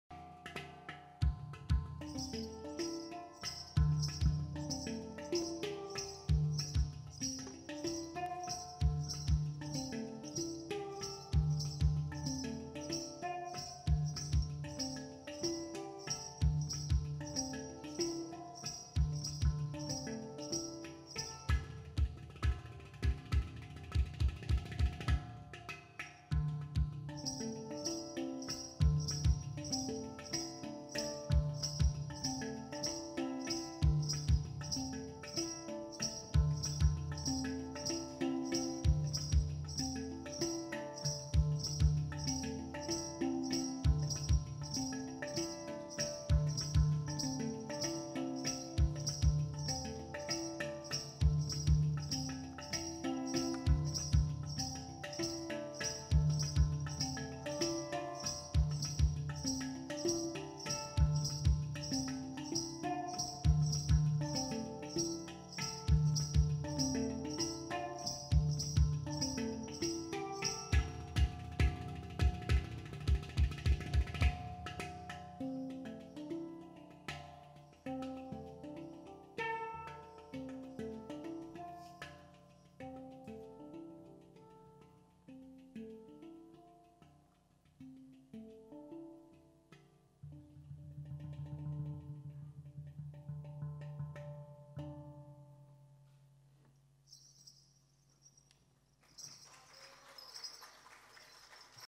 Medina Swing - Solo Khalid el Berkaoui
Medina Swing en concert a la Fol du 20 Octobre 2018
Percussions : Khalid Berkaoui

Contact et Booking :
WhatApp : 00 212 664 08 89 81